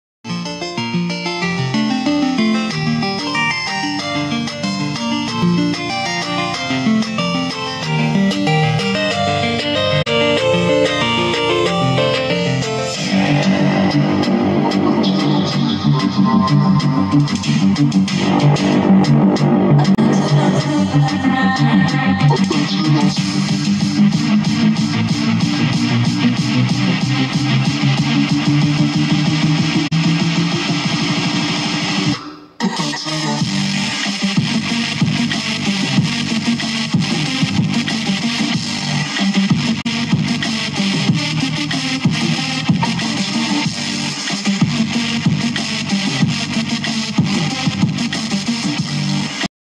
Vintage Hi-Fi Panasonic Thrusters 3 way speakers.
Vintage Hi-Fi Panasonic Thrusters 3 way speakers. Very loud.  Tested on a Yamaha Amp.